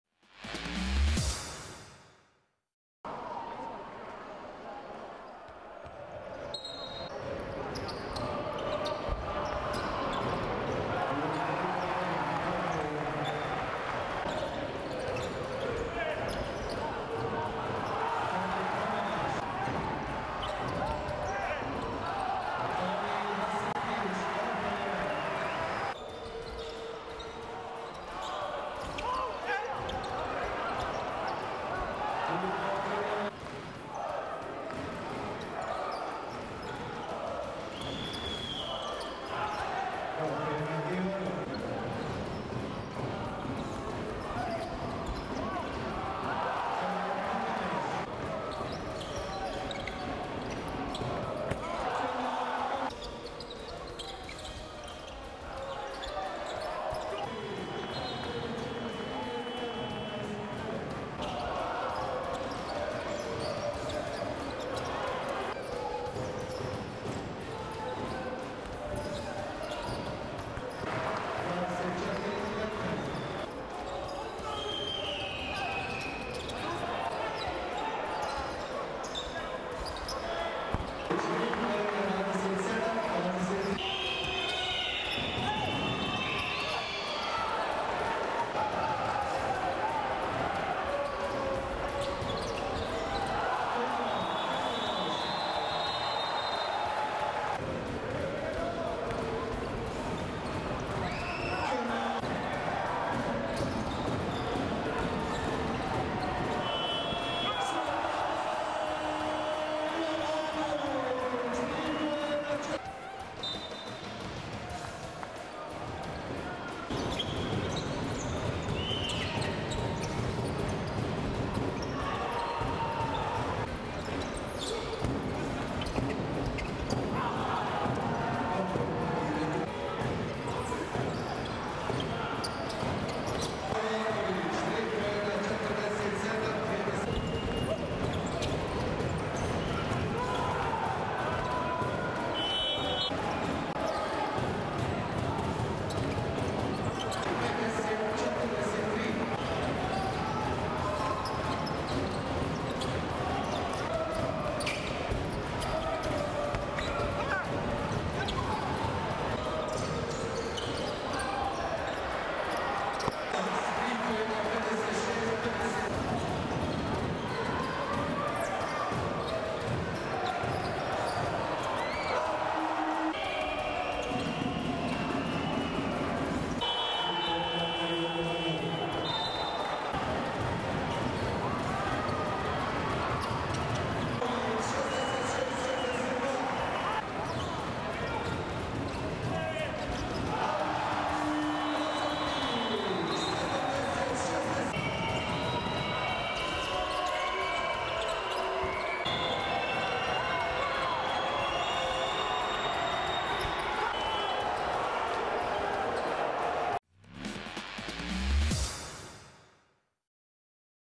2019 ABA 2 Playoffs highlights, Game 1: Spars - MZT Skopje Aerodrom (20.3.2019)
Take a look at the highlights of the 2019 ABA 2 Playoffs Game 1 between Spars and MZT Skopje Aerodrom in Novo Sarajevo Hall, Sarajevo.